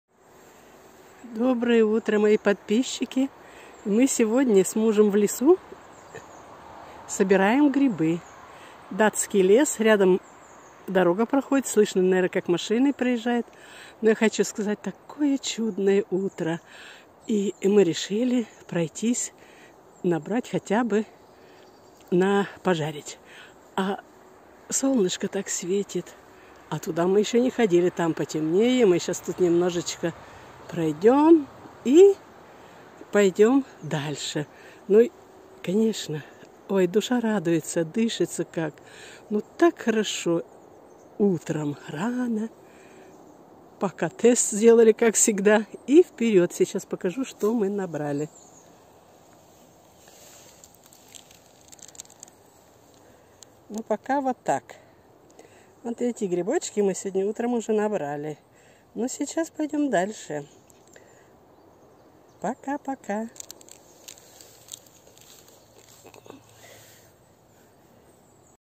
1 сентября 2021 г.Солнечный лес и грибы.
Приглашаю в солнечный лес по грибы. В Данию.